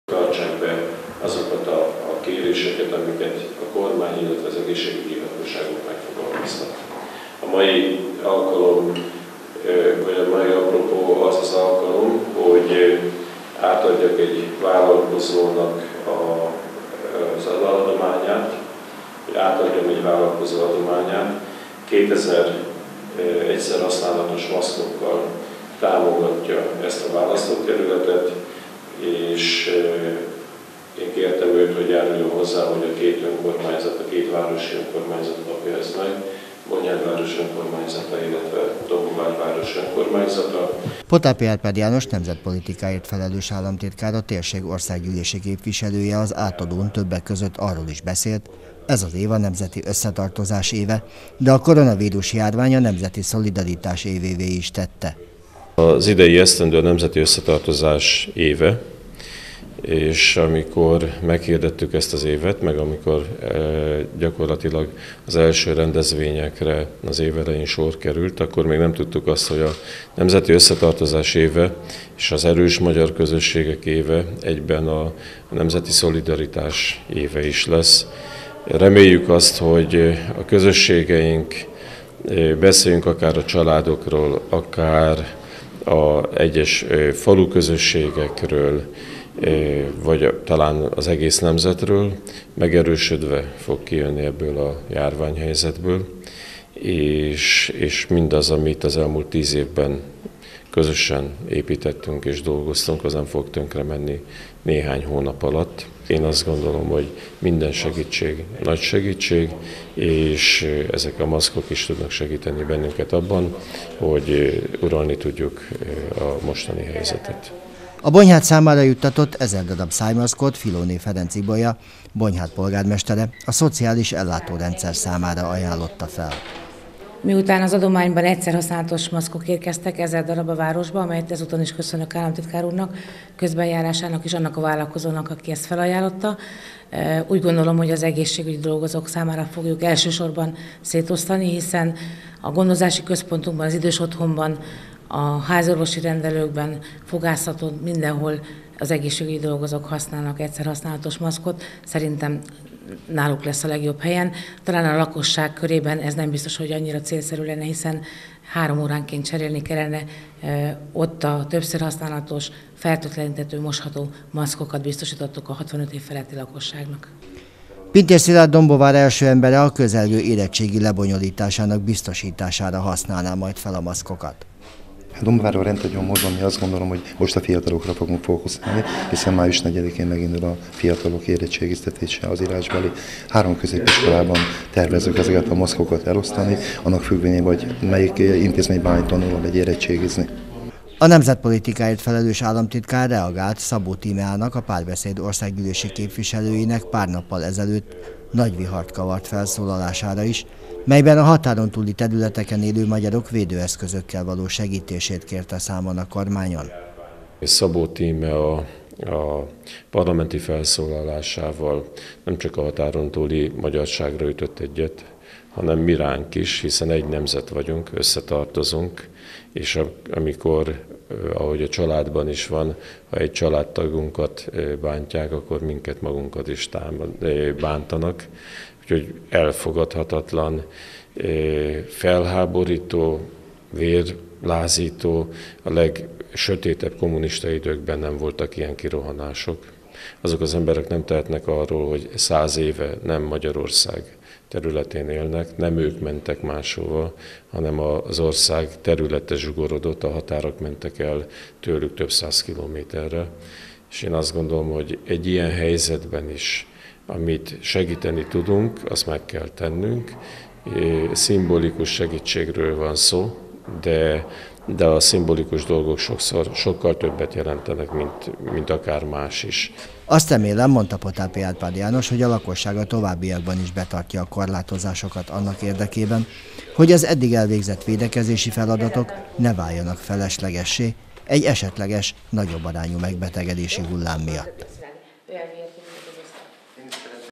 Kétezer szájmaszkot adott választókerülete két városának Potápi Árpád János országgyűlési képviselő
Kétezer szájmaszkot adott át választókerülete két városának Potápi Árpád János országgyűlési képviselő, nemzetpolitikáért felelős államtitkár. Az adomány egy vállalkozó ajándéka volt. Bonyhád az egészségügyi ellátórendszert, míg Dombóvár az érettségi zavartalan lebonyolítását támogatja az egyszer használatos védőfelszereléssel.